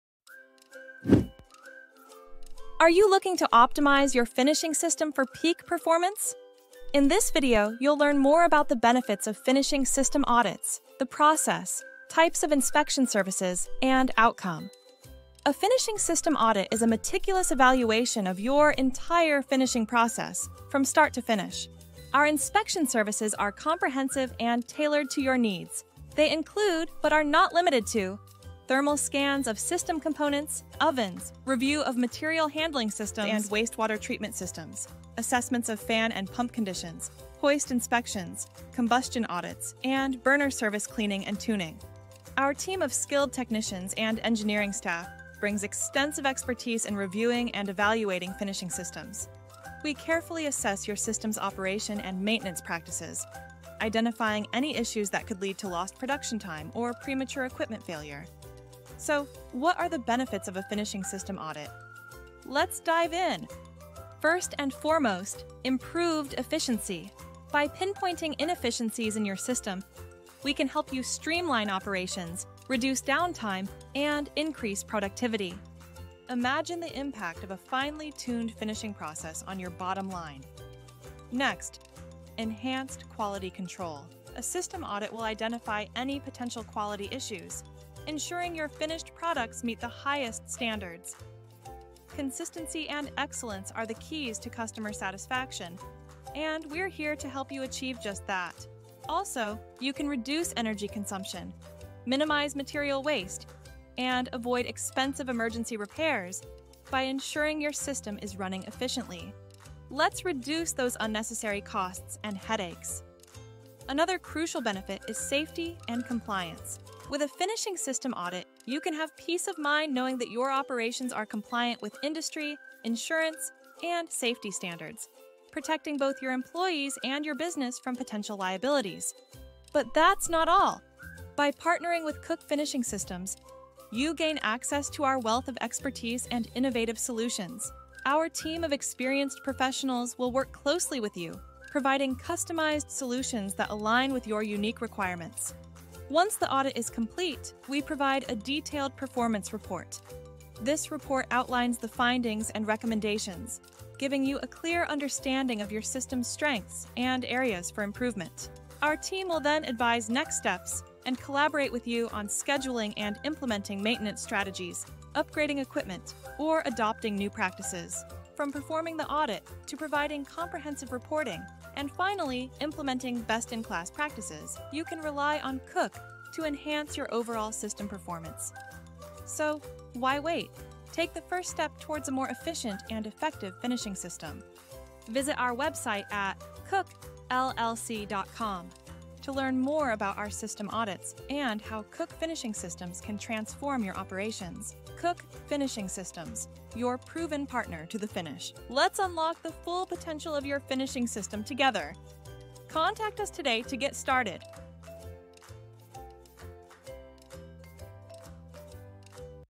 KOCH System Audits Explainer Video
Welcome to our System Audits Explainer Video! In this video, we delve into the importance of regular maintenance and inspections for your finishing equipment. A finishing system audit is a comprehensive evaluation that assesses your equipment's operation and maintenance practices. Conducted by our experienced technicians, this audit identifies existing and potential issues while providing actionable recommendations for improvement.

A finishing system audit offers numerous benefits. It enhances performance by optimizing your equipment's efficiency, leading to increased productivity and improved product quality. Additionally, it extends the lifespan of your equipment by preventing premature failures and costly replacements through early issue detection. Cost savings are also a significant advantage, as optimizing performance can reduce energy consumption, minimize material waste, and help avoid expensive emergency repairs. Furthermore, system audits ensure your finishing system meets safety regulations and industry standards, protecting both your employees and your business from potential liabilities.

At KOCH Finishing Systems, we offer complete and partial system audits tailored to all finishing equipment. Our skilled technicians bring extensive expertise to review your systems and provide a detailed performance report with findings and recommendations. Learn how we can collaborate to enhance your system's performance and longevity!